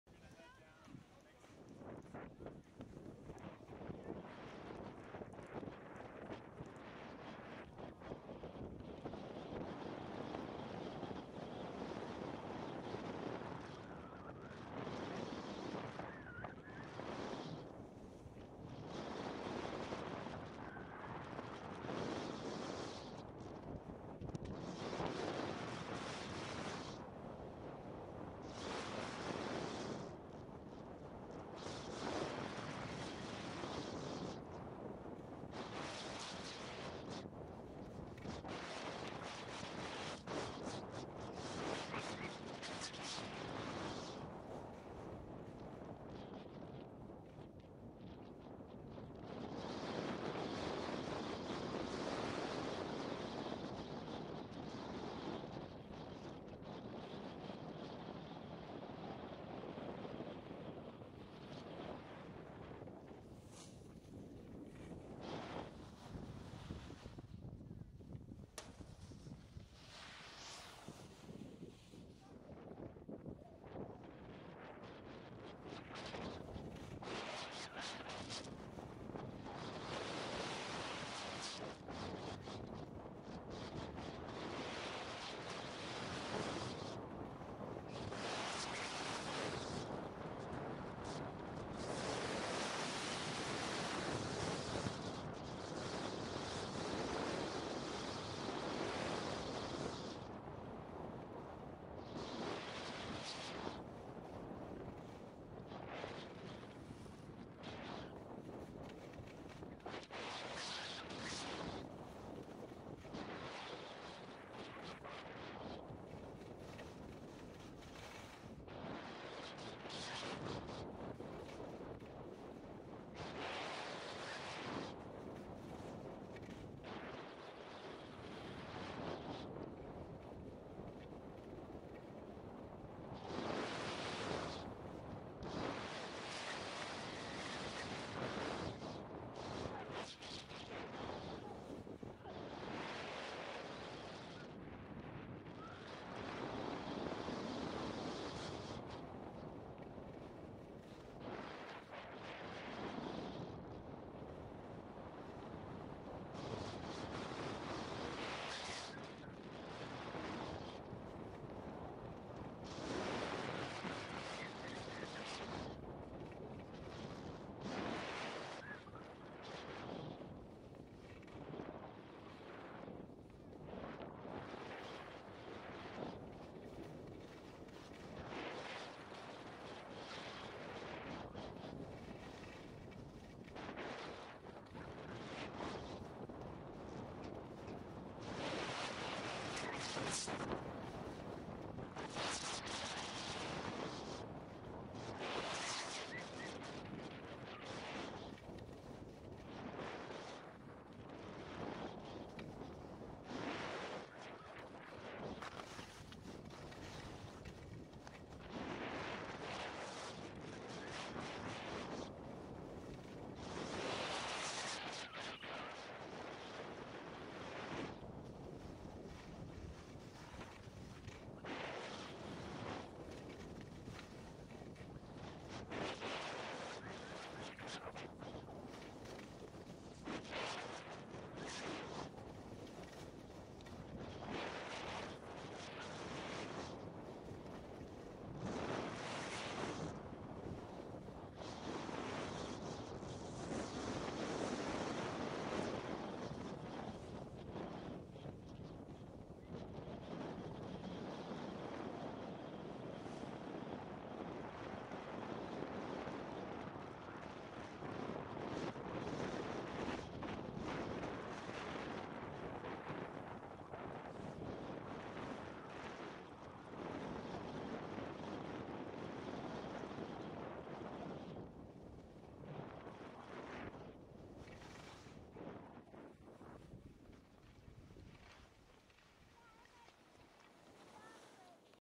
Keystone Paymaster Full-Trail 360
A full 4K 360° VR compatible video of Keystone Ski Resort. This blue run is called Paymaster and is located on the West side of Dercum Mountain. This run starts on Schoolmarm then a right turn takes you onto Paymaster, the run ends at the Montezuma lift.

Start Elevation: 11,628 ft.
Distance: 1.7 mi.
Date: 1/14/2018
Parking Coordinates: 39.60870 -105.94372